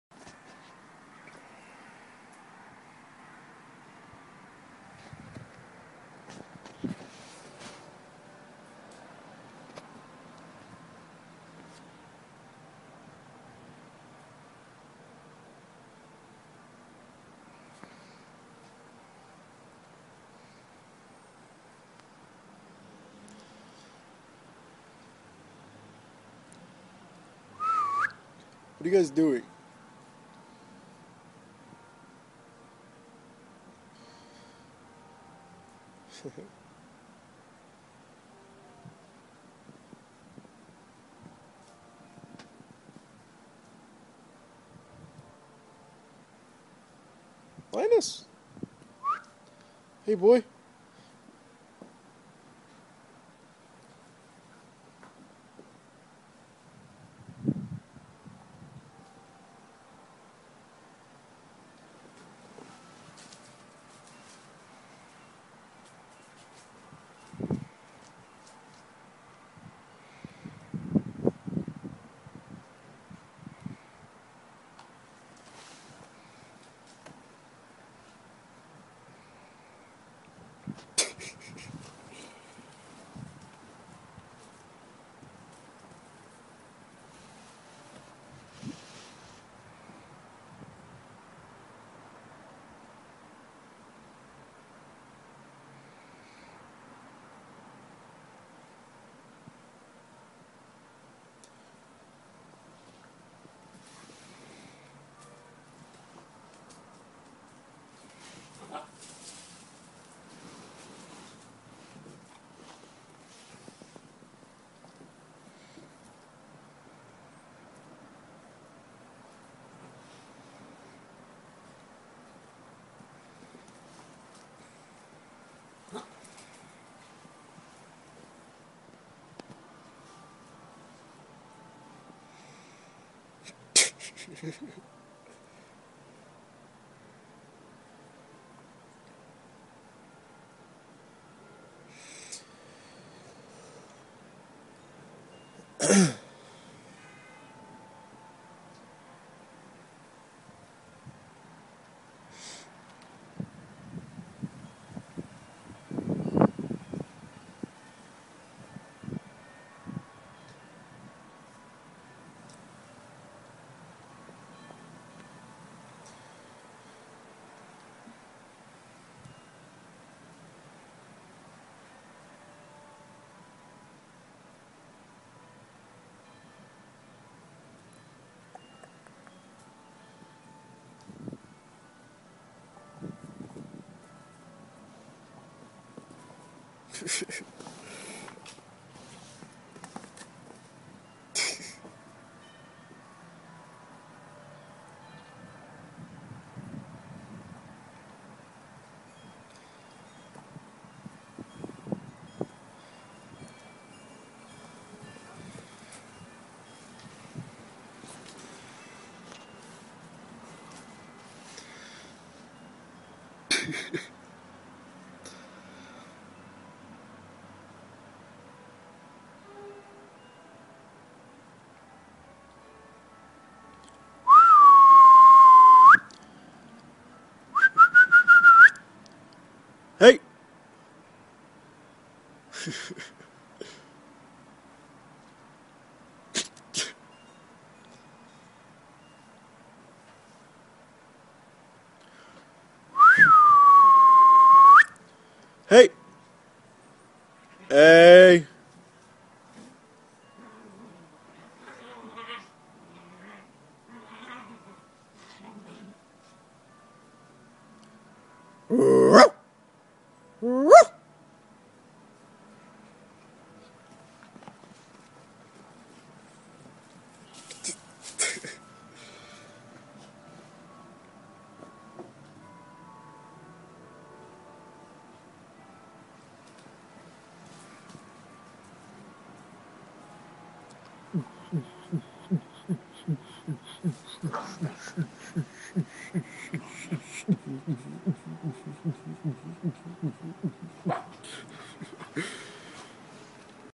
Suki and Linus playing in the backyard.
My two marvelous little pups having a go out in the yard.